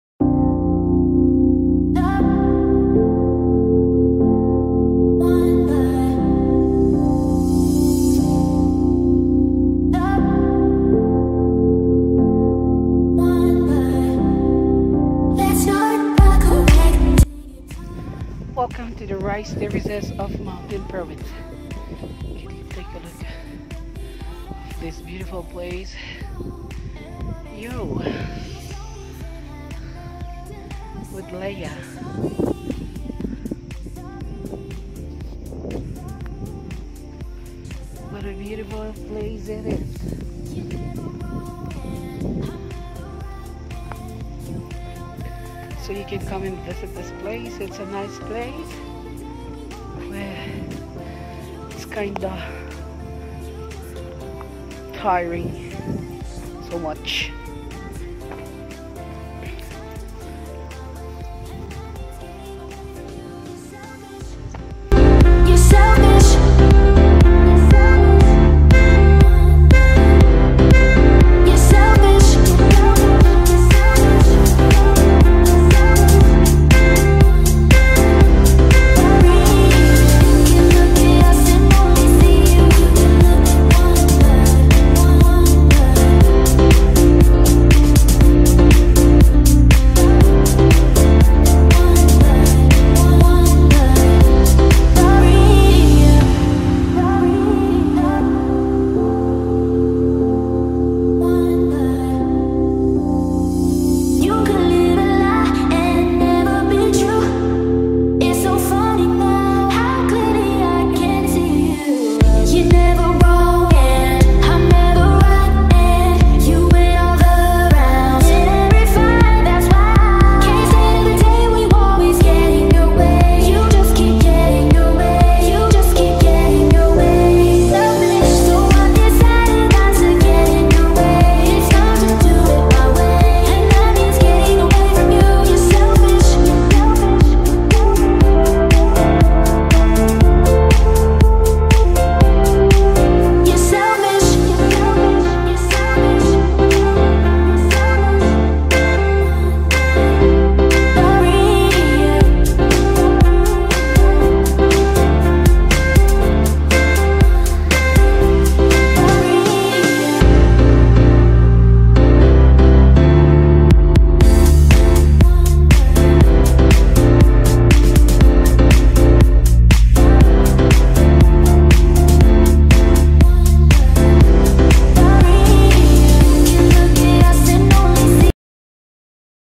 SUNNY SIDE'S BEAUTIFUL SPOT (Mountain Province)
Located at Tadian Mountaon Province.

Visit the place now!

Mount Mogao is a good place to climb also.
so refreshing there.